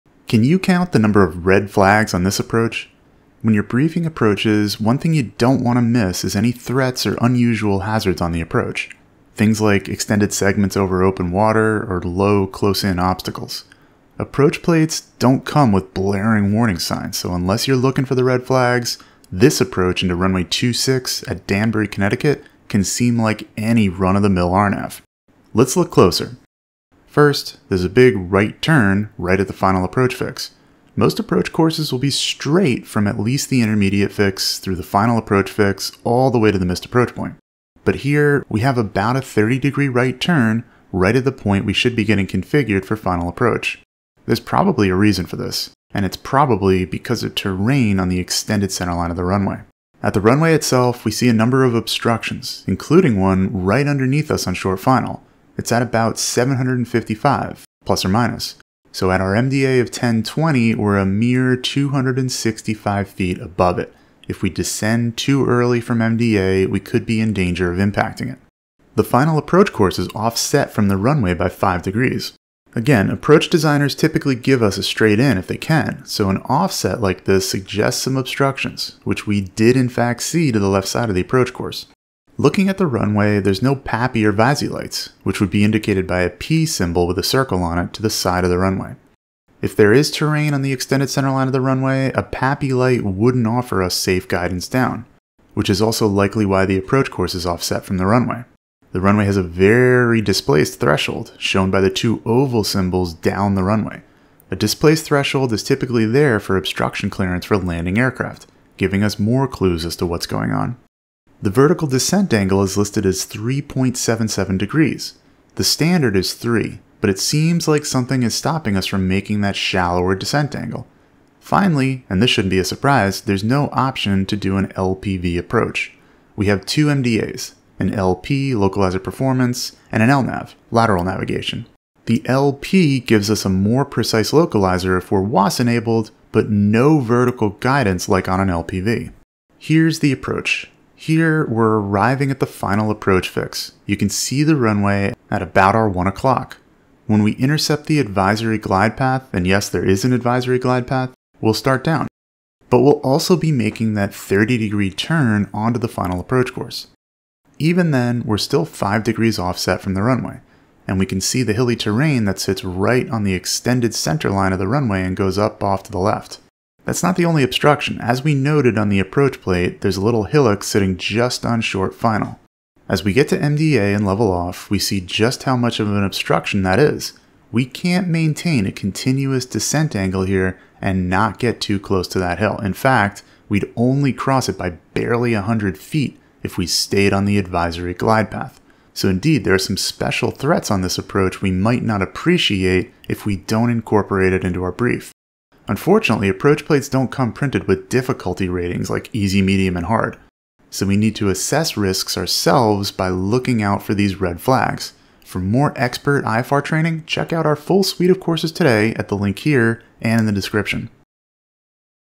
Look Closely at this Approach | Briefing Instrument Approaches
There are a number of red flags on this approach making it more difficult than many others like it. Let's dive into the details on this approach plate as we brief it.